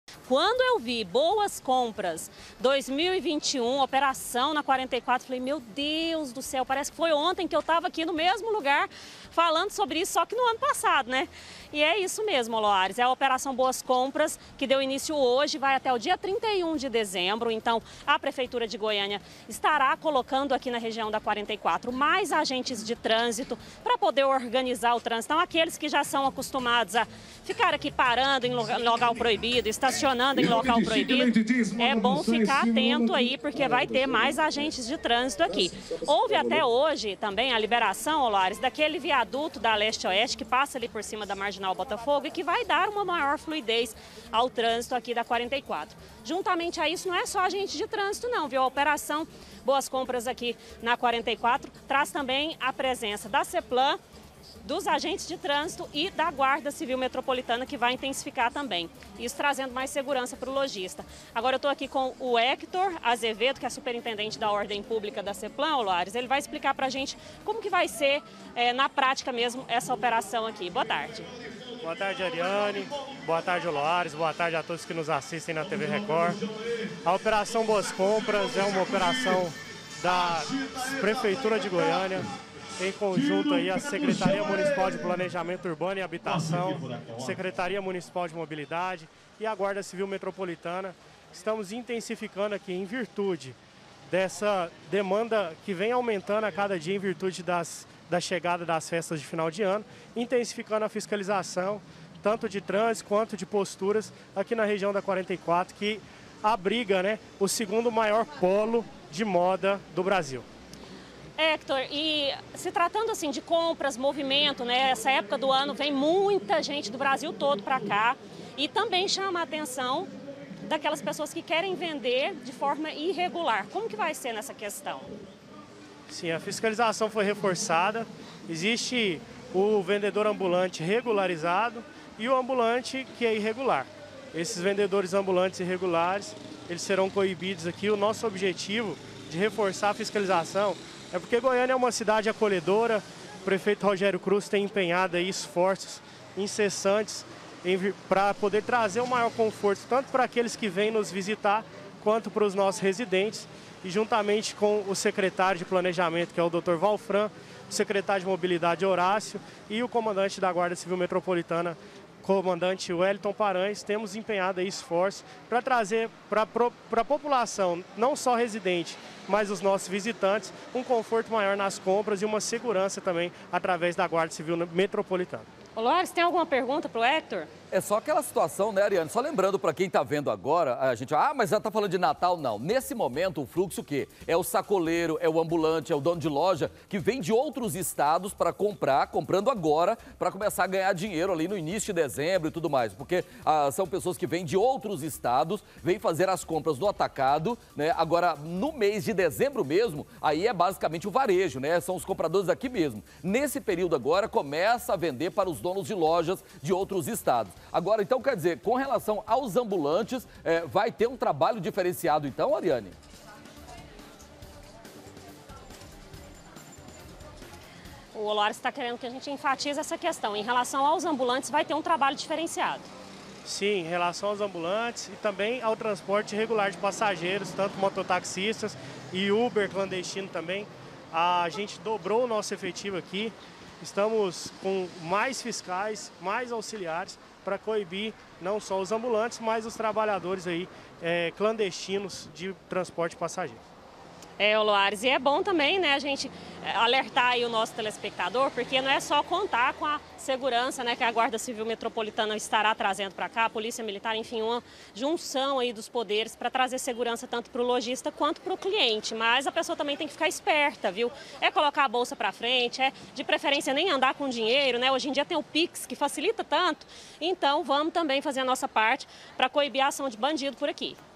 OPERAÇÃO BOAS COMPRAS: CLIENTES E COMECIANTES MAIS SEGUROS NA 44
COMEÇA OUTUBRO E JÁ SE FALA EM NATAL. PELO MENOS PARA O COMÉRCIO. FOI LANÇADA HOJE A OPERAÇÃO BOAS COMPRAS NA REGIÃO DA 44. UM ESQUEMA ESPECIAL PRA GARANTIR UM FIM DE ANO MAIS SEGURO PRA CLIENTES E COMERCIANTES.//